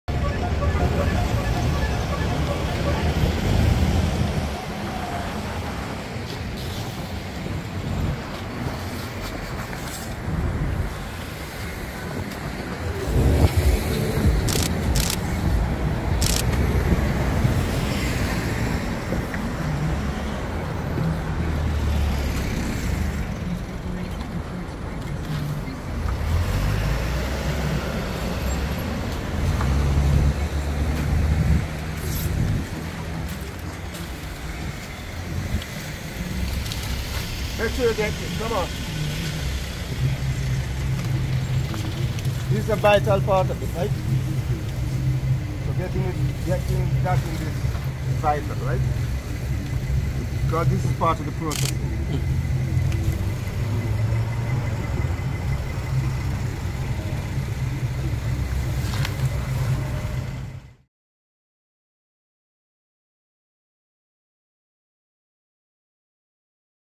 Nation Update: Alex the broom man
Just call him Alex the broom man!
He has become a fixture along the busy junction of Collymore Rock and Pine Road, St Michael, where he makes and sells straw brooms.
Pointing out that broom-making was a popular tradition in Jamaica, where he was born, Alex admitted he learnt the trade here.
“I happened to meet up with this man who makes brooms and by talking and talking I got involved too,” he said, smiling.
Alex said while the broom, which is made from palm leaves, was not losing its history, there was a need for more people to become involved in making it. (Video by Reco Moore)